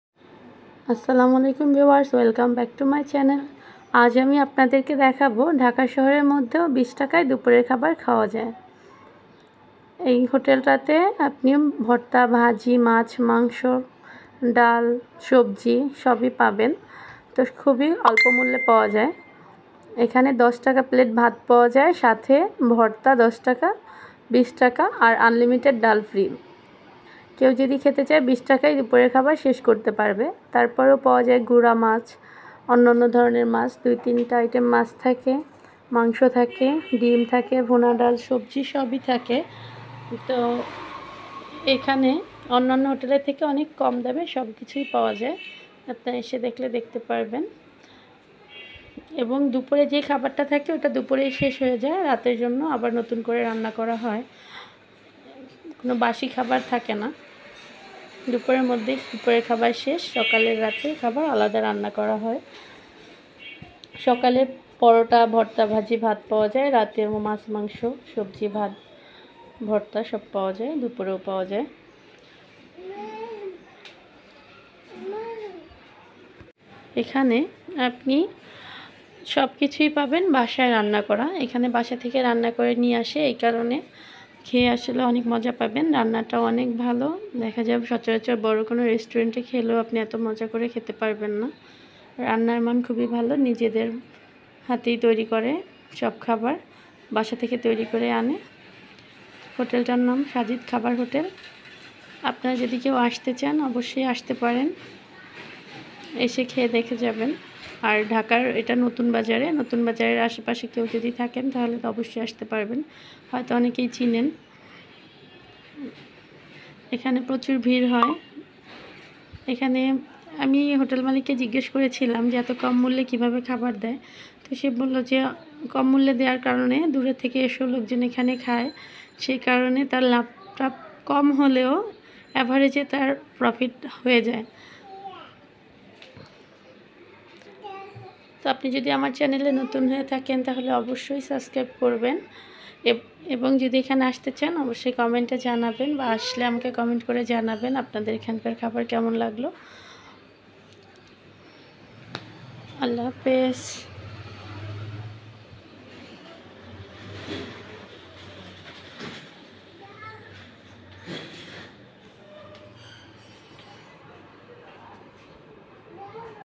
২০ টাকায় দুপুরের খাবার 😱😱ঢাকা শহরে
মাত্র 20 টাকায় দুপুরের খাবার তাও আবার ঢাকা শহরে কি অবাক হলেন। অবাক হওয়ারই কথা বর্তমানে দ্রব্যমূল্যের ঊর্ধ্বগতি এই সময়ে এত কম দামে খাবার খাওয়া আসলেই অবিশ্বাস্য। নতুন বাজার 100 ফিট রোড ফরাজী হসপিটাল এর অপজিটে সাজিদ খাবার হোটেল। এই হোটেলে ভর্তা ভাজি মাছ মাংস শাকসবজি সব পাওয়া যায়। খুবই কম মূল্যে পাওয়া যায়।